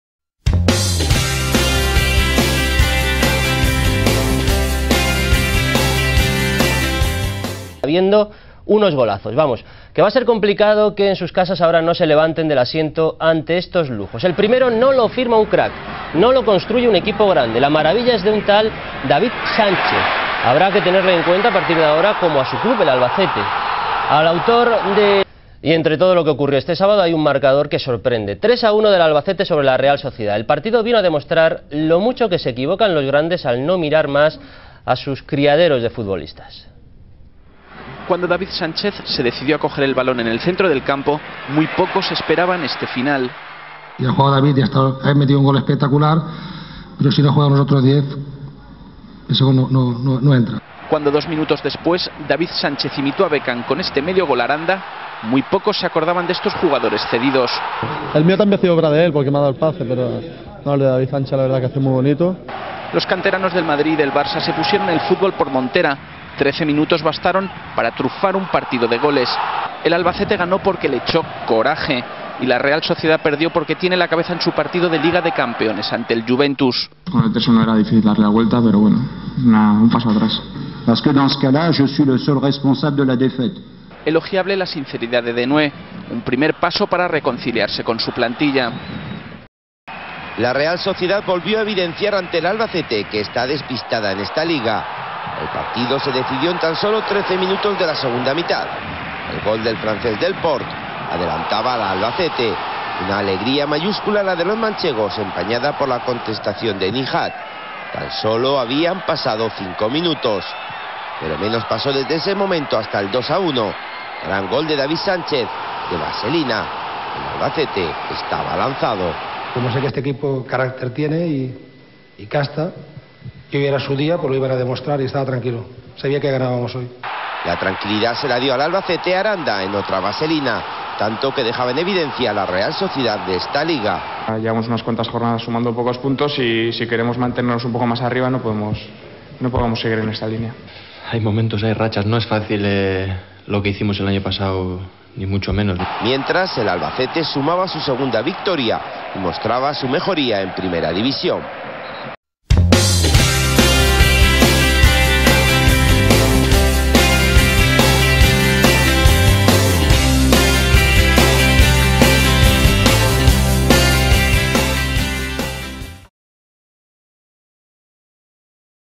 Albacete 3 - Real Sociedad 1. Temp. 03/04. Jor. 7.
Albacete. Estadio Carlos Belmonte. 18 de octubre de 2003. Goles: Delporte (1-0), Nihat (1-1), David Sánchez (2-1), Aranda (3-1).